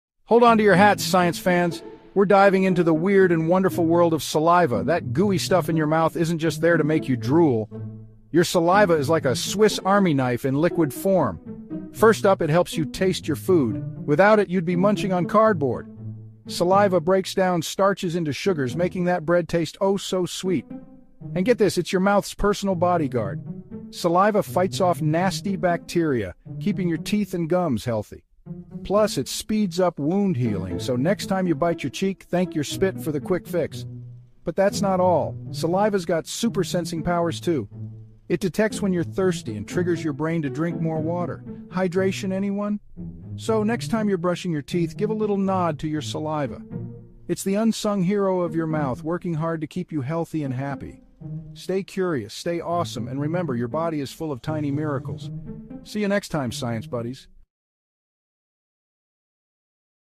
The Amazing Work Of Saliva In Your Mouth
Discover the fascinating world of saliva in our animated short, "Why Your Saliva is a Superhero!" In just a few minutes, we’ll dive into the crucial roles saliva plays in digestion, oral health, and protecting your body. Our vibrant 3D animations, inspired by Disney Pixar’s charm, will bring these important functions to life with bold, popping subtitles for an engaging viewing experience. Narrated by a friendly male voice with a Californian accent, this video is not just informative; it’s fun! Join us on this remarkable journey and learn why saliva is more than just spit—it's a superhero for your health!